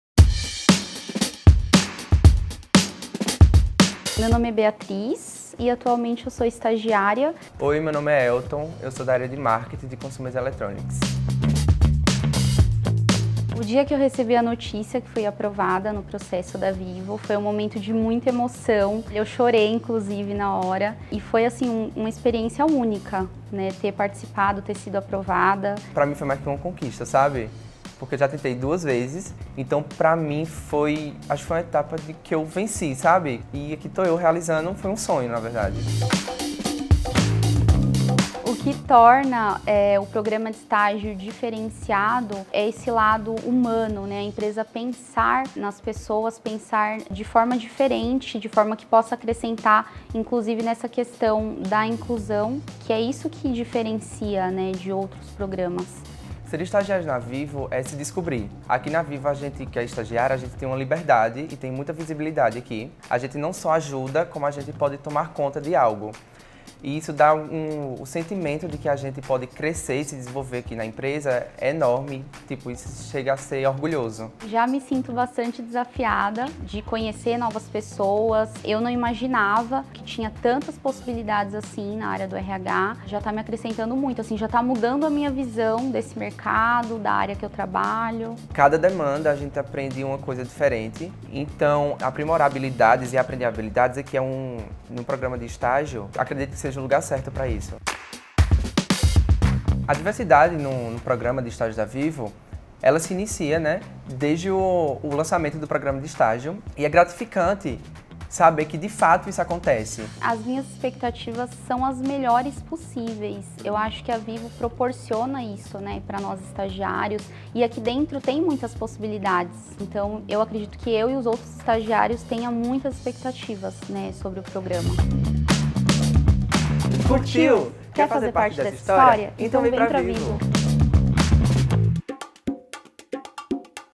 Ep.2 Websérie Programa de Estágio Vivo 2023.3
Foi aqui que pediram conexão para transformar carreiras? Então vem dar play no 2º episódio da Websérie | Programa de Estágio Vivo 2023 - 2º Semestre!

Hoje vamos conversar com dois estagiários, Elton e Beatriz, para nos contarem um pouquinho sobre essa jornada como novos VivoLovers.

Fala a verdade, já tá se imaginando fazendo parte desse time, né? Então não perca tempo e faça a sua inscrição